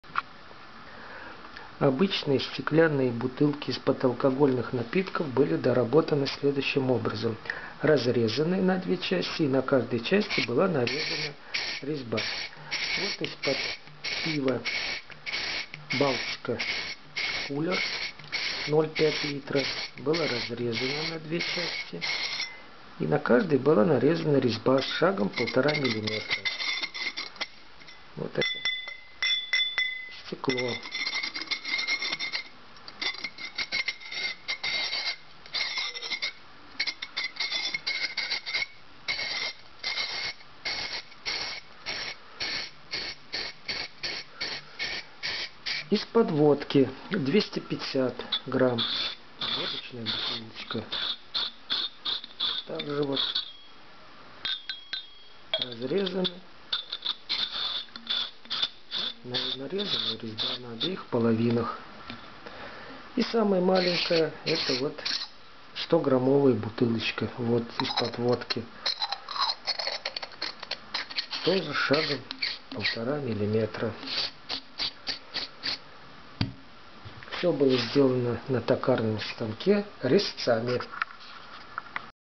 РЕЗЬБОВОЕ СОЕДИНЕНИЕ СТЕКЛЯННЫХ БУТЫЛОК
Показано резьбовое соединение стеклянных бутылок. Стеклянные бутылки из-под алкогольных напитков были разрезаны на токарном станке  на 2 части, и на каждой части нарезана резьба с шагом 1,5 мм. Бутылки стали разборными. Бутылки объёмом 0,1, 0,25 и 0,5 литра. Всё было сделано на токарном станке резцами.
Шары с выточенными внутри многогранниками (тела Платона) и шароточки для вытачивания шаров на токарном станке можно посмотреть